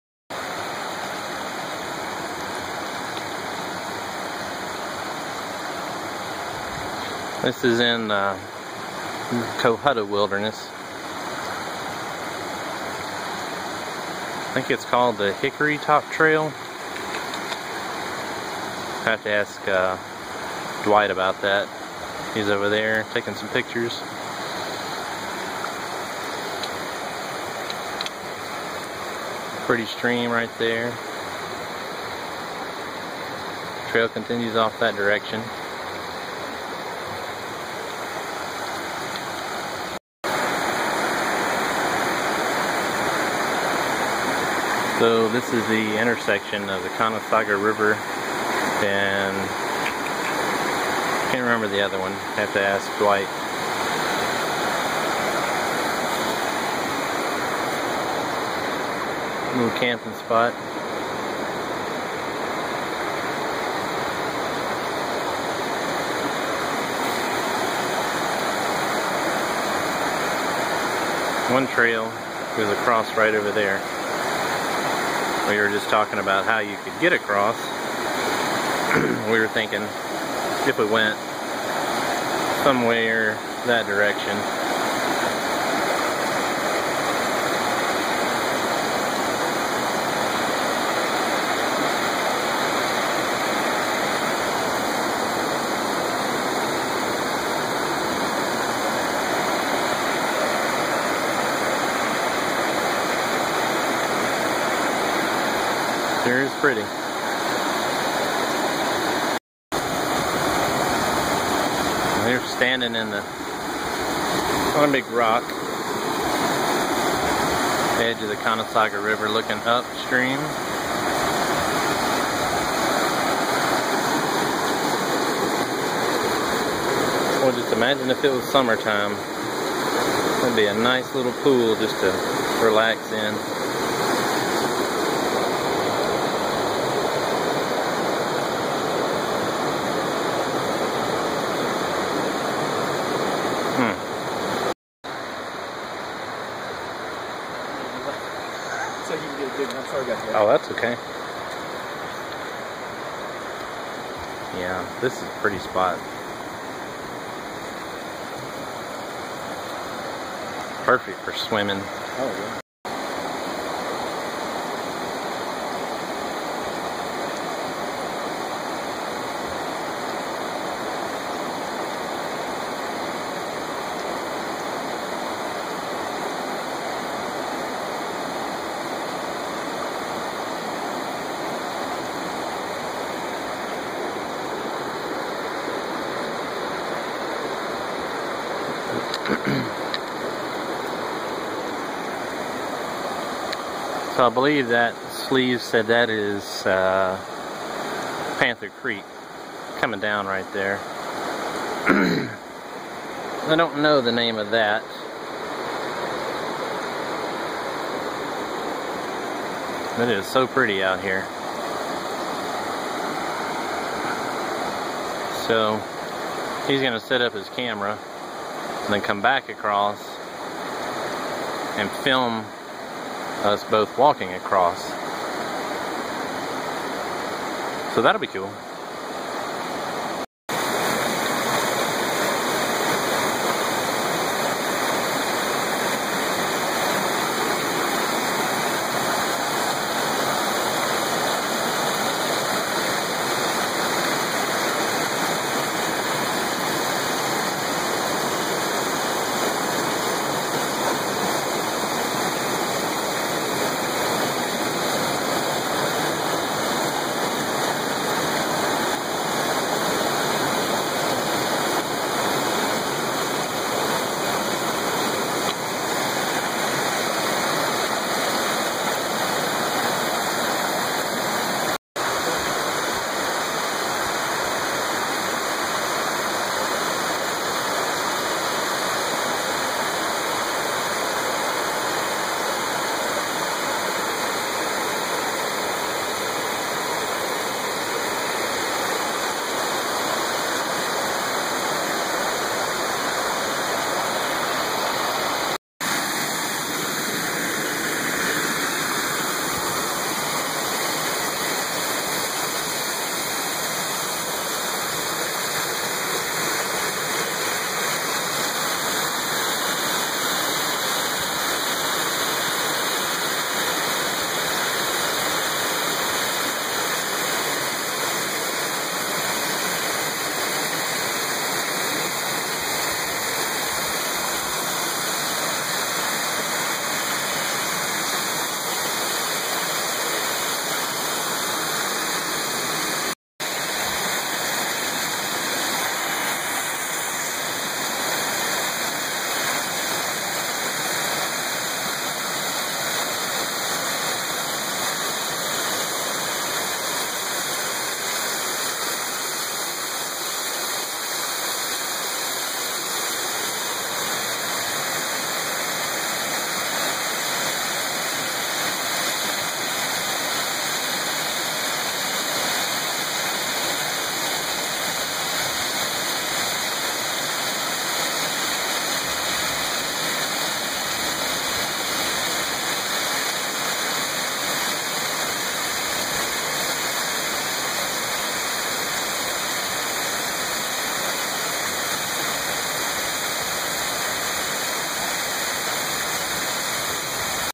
Cohutta Wilderness hike with Sleeves 3.18.16
This was an overnight hiking/camping trip taken in the Cohutta Wilderness with Sleeves.  His video will be much better than mine, but this is my first attempt at combining multiple clips to create one video.  Next steps are experimenting with cutting clips and adding captions.